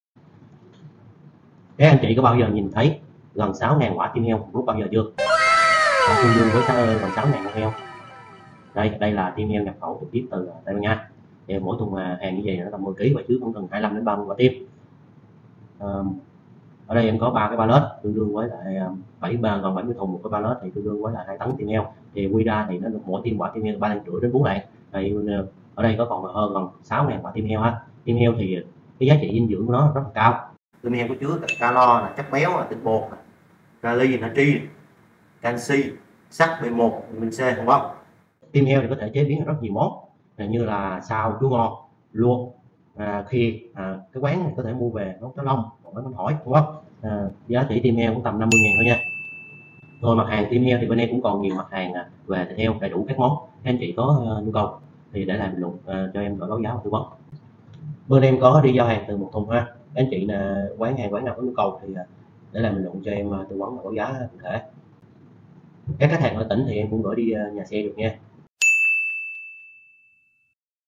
Tim heo nhập khẩu
Tim heo nhập khẩu giá rẻ cho các quán ăn, nhà hàng, khách sạn